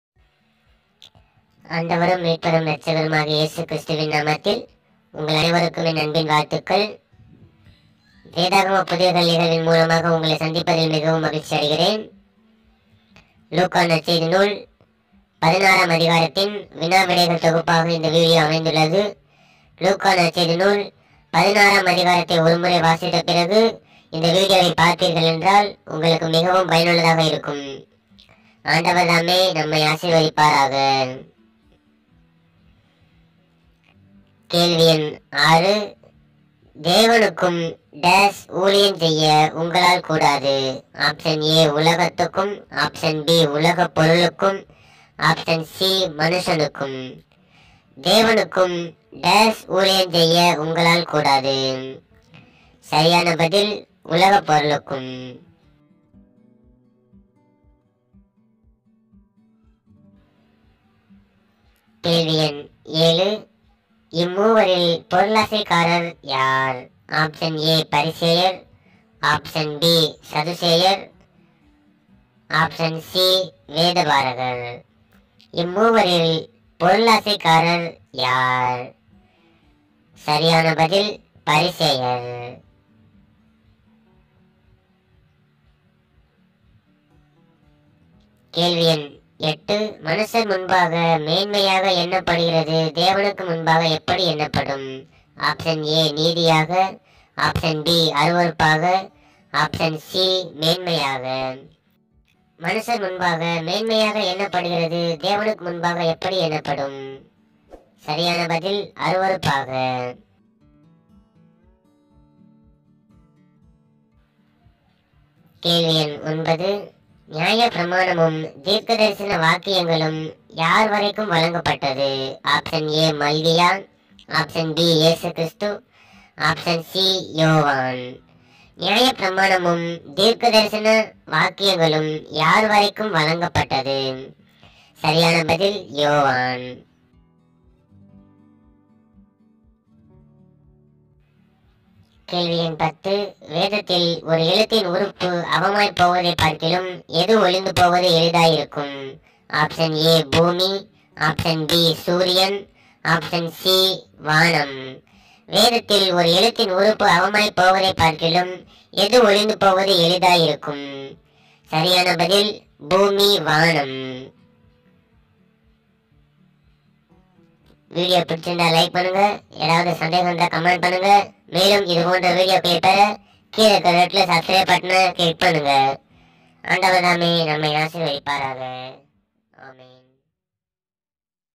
Luke 16 Quiz நியாயப்பிரமாணமும் தீர்க்கதரிசன வாக்கியங்களும் யார் வரைக்கும் வழங்கப்பட்டது? | Jesus sam
My Telegram Group 👉: Jesus & Sam

லூக்கா நற்செய்தி நூல் அதிகாரம் பதினாறு வினா விடைகள் தொகுப்பு பாகம் இரண்டு

06. தேவனுக்கும் ________ ஊழியஞ்செய்ய உங்களால் கூடாது?
     A) உலகத்துக்கும்
     B) உலகப்பொருளுக்கும்
     C) மனுஷனுக்கும்

07. இம்மூவரில் பொருளாசைக்காரர் யார்?
     A) பரிசேயர்
     B) சதுசேயர்
     C) வேதபாரகர்

08. மனுஷர் முன்பாக மேன்மையாக எண்ணப்படுகிறது தேவனுக்கு முன்பாக எப்படி எண்ணப்படும்?
     A) நீதியாக
     B) அருவருப்பாக
     C) மேன்மையாக

09. நியாயப்பிரமாணமும் தீர்க்கதரிசன வாக்கியங்களும் யார் வரைக்கும் வழங்கப்பட்டது?
     A) மல்கியா
     B) இயேசு கிறிஸ்து
     C) யோவான்

10. வேதத்தில் ஒரு எழுத்தின் உறுப்பு அவமாய் போவதைப் பார்க்கிலும் எது ஒழிந்துபோவது எளிதாய் இருக்கும்?
     A) பூமி
     B) சூரியன்
     C) வானம்